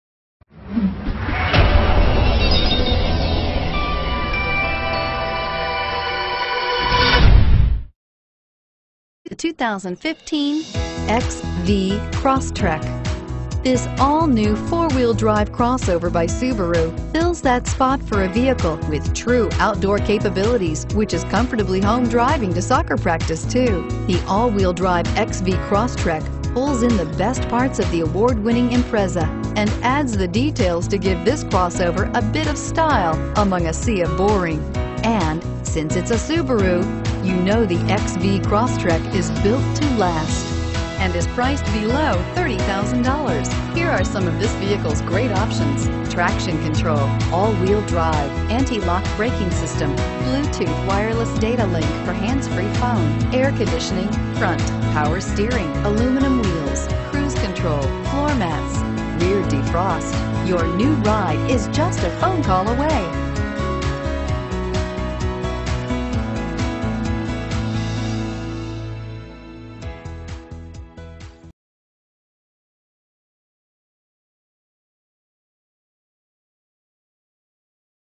2015 Subaru XV Crosstrek Ventura CA 286715
2015 Subaru XV Crosstrek

Kirby Subaru
6404 Auto Center Drive

Looking for the right SUV? Today could be your lucky day. With 0 miles, this Dark Gray, 2015 Subaru XV Crosstrek equipped with Automatic transmission could be yours. Request more information and set up a test drive right away.